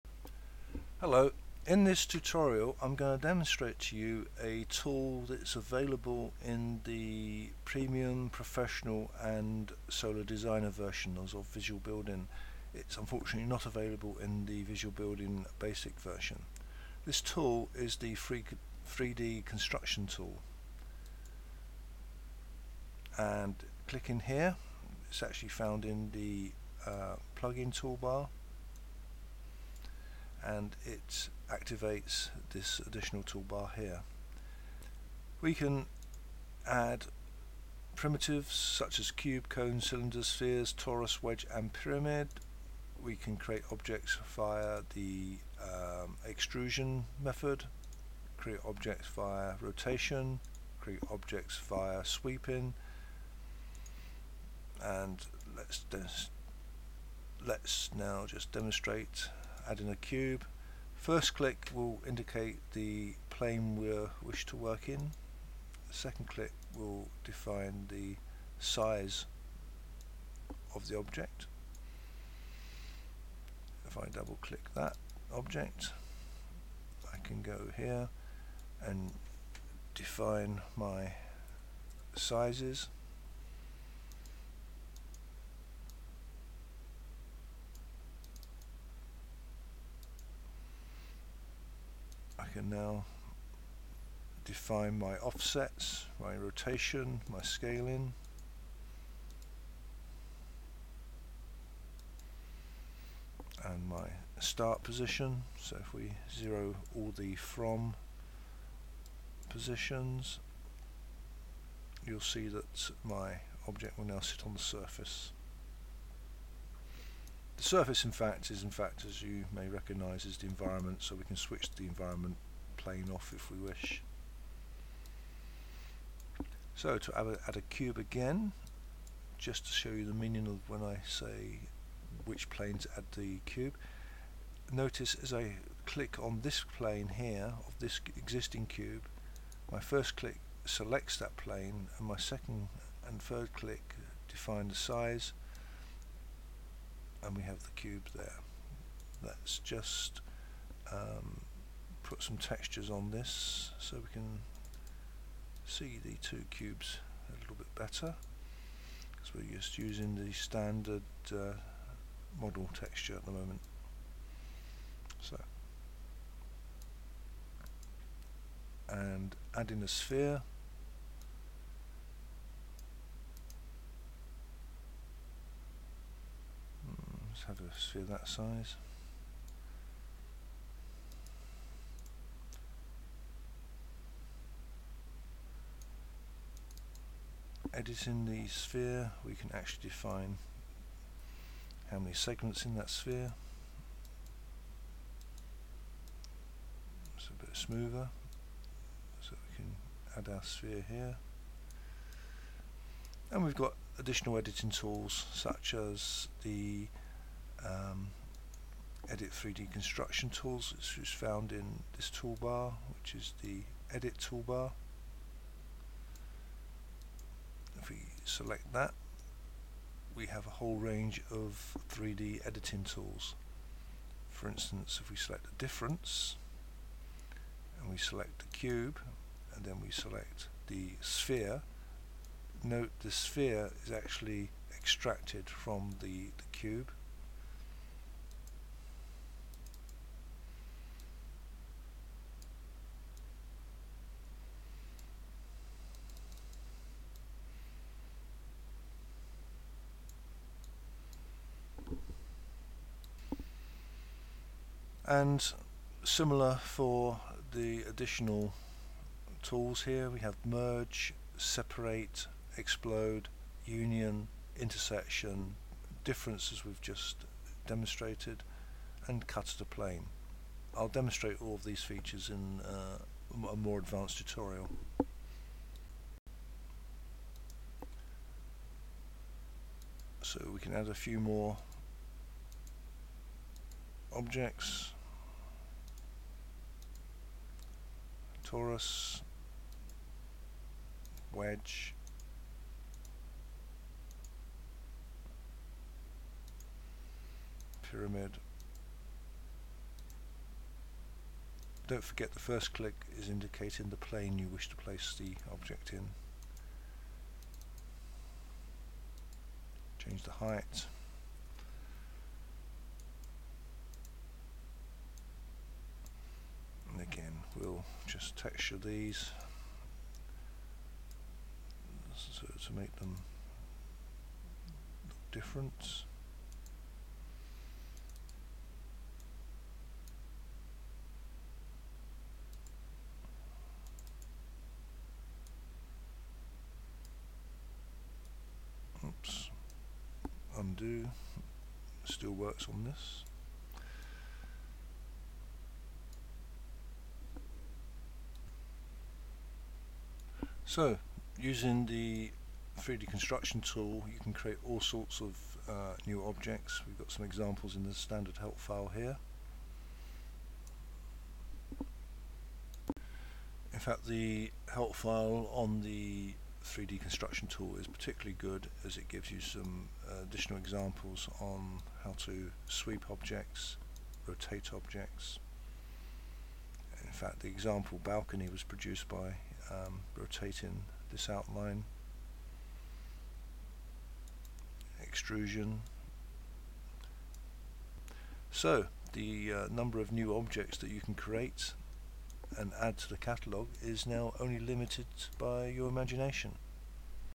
Visual Building Tutorial: 3D Model Construction
This tutorial introduces the use of the 3D modelling tools found in Visual Building Professional, Premium and Solar Designer.

Visual Building is a building / house design software application, that will enable you to design your own home producing quality floorplans and 3D Visualisations.